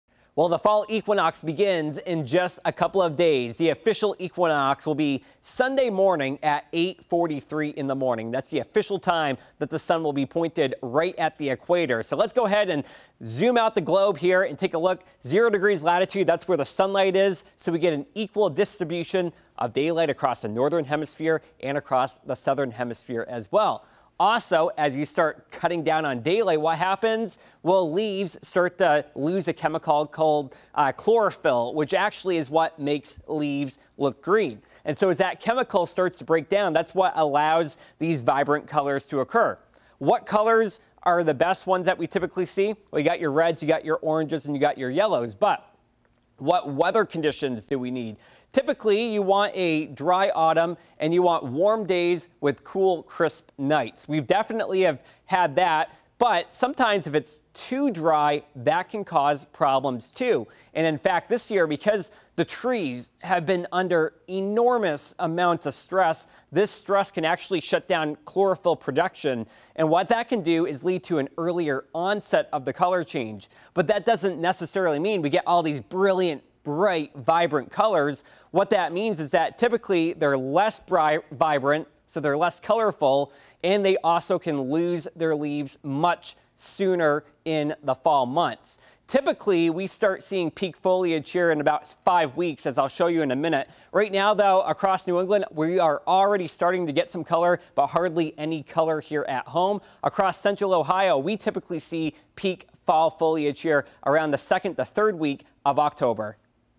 Extreme drought could lead to early fall foliage in central Ohio
Average peak foliage in central Ohio is Oct. 15-29, but this year it may be earlier.